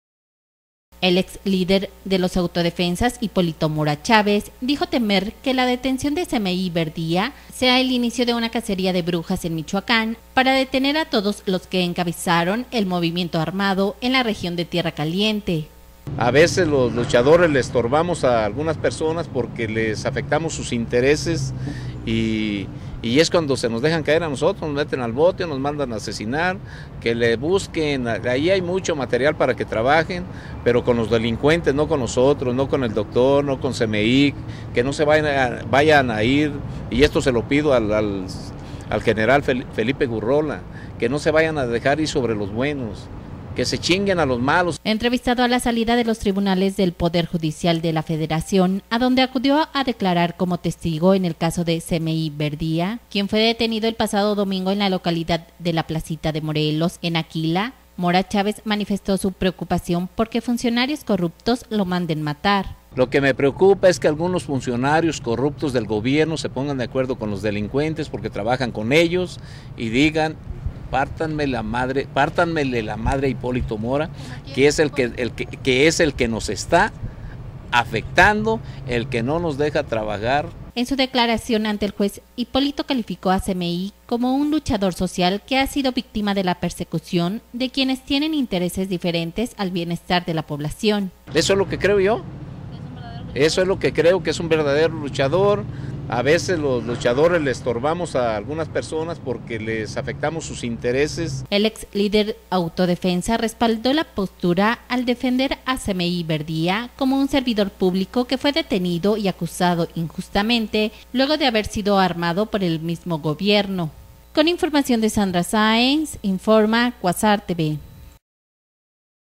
Teme Hipólito Mora que la detención de Cemeí sea una cacería de brujas
El ex líder de los autodefensas, Hipólito Mora Chávez, dijo temer que la detención de Cemeí Verdeí sea el inicio de una cacería de brujas en Michoacán para detener a todos los que encabezaron el movimiento armado en la región de tierra caliente...